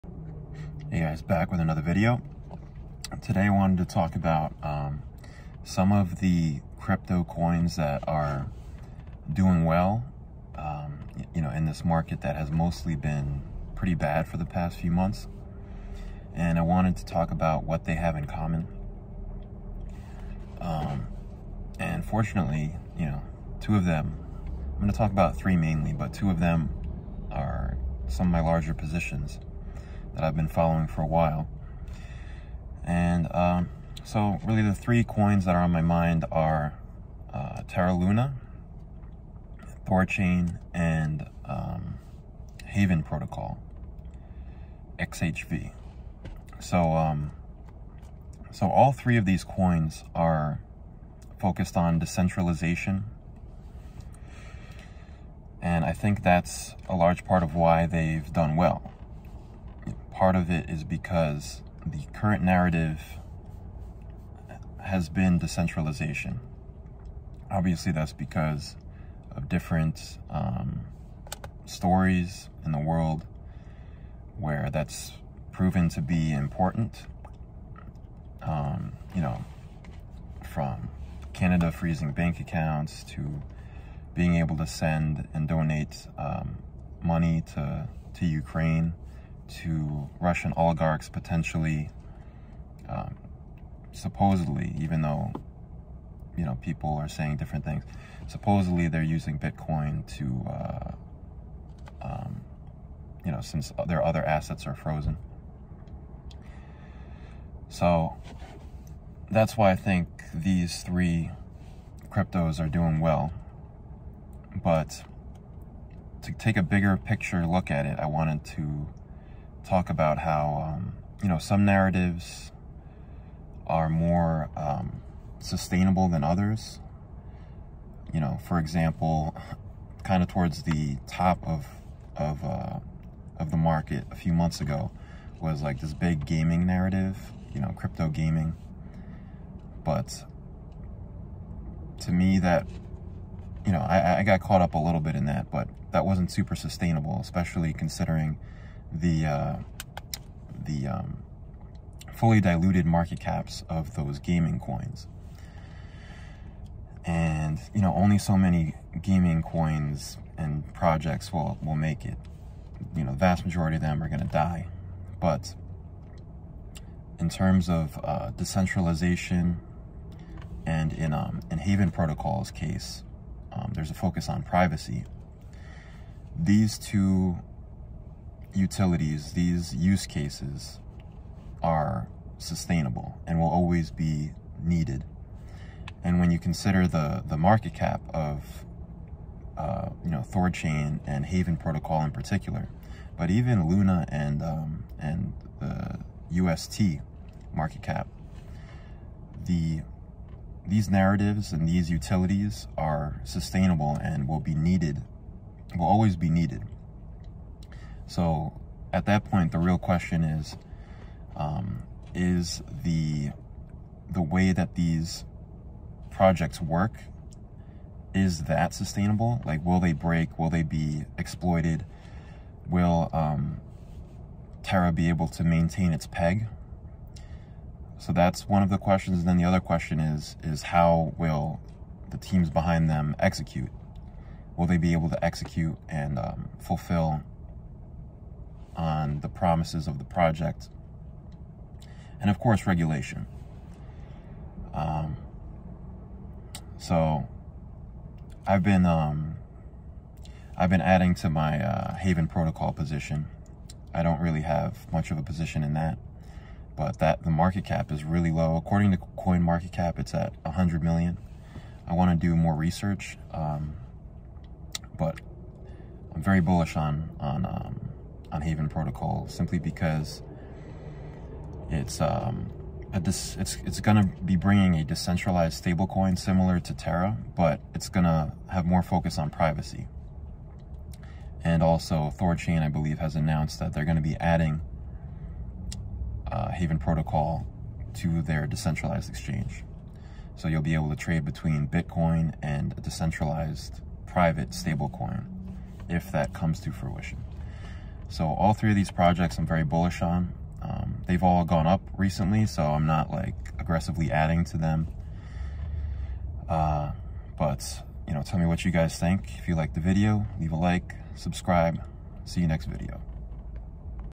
3 Coins I Am Holding Long Term! How I Think About Narratives
I talk about Terra Luna (LUNA), Thorchain (RUNE), and Haven Protocol (XHV). None of my videos are financial advice